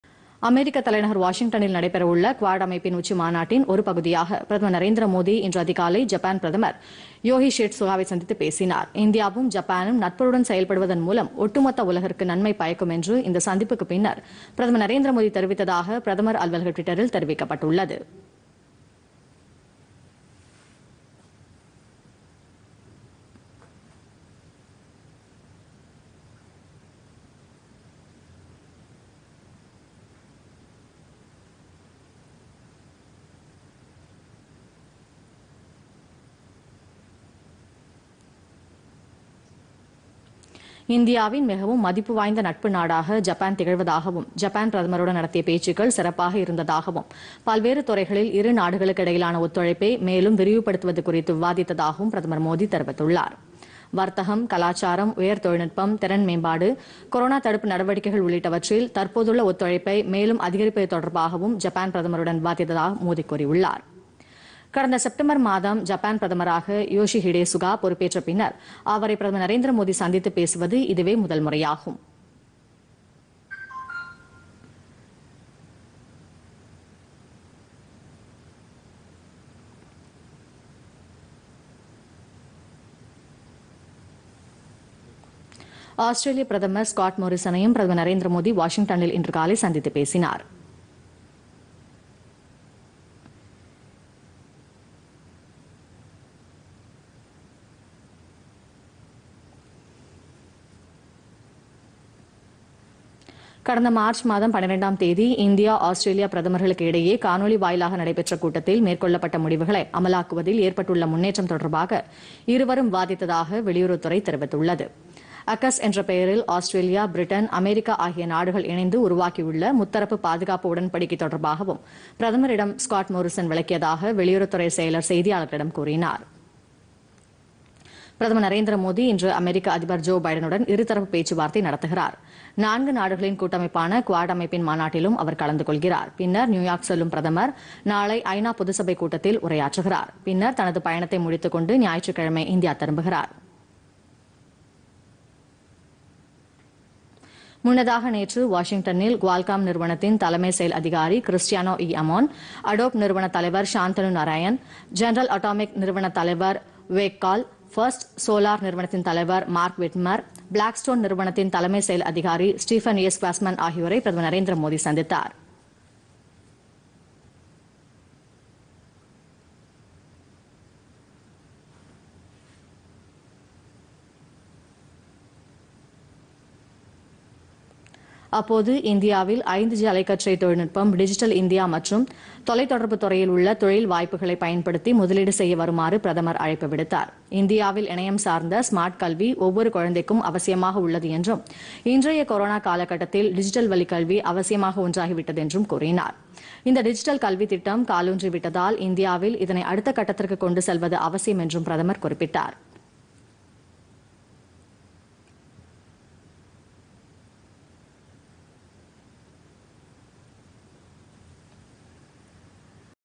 ஜப்பான், ஆஸ்திரேலியா பிரதமர்களையும் சந்தித்தார் மோதி
பிரதமர் நரேந்திர மோதி, ஜப்பான் பிரதமர் யோசிஹிடே சுகா மற்றும் ஆஸ்திரேலிய பிரதமர் ஸ்காட் மோரீசனையும் சந்தித்து பேசினார்